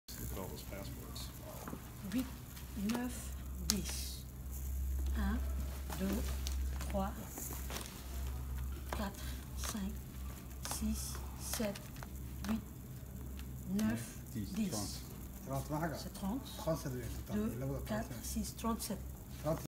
Counting Passports, As One Does In Djibouti
Hanging out in the customs clearance area at Djibouti International Airport while our host counts a stack of passports.  A bit surreal.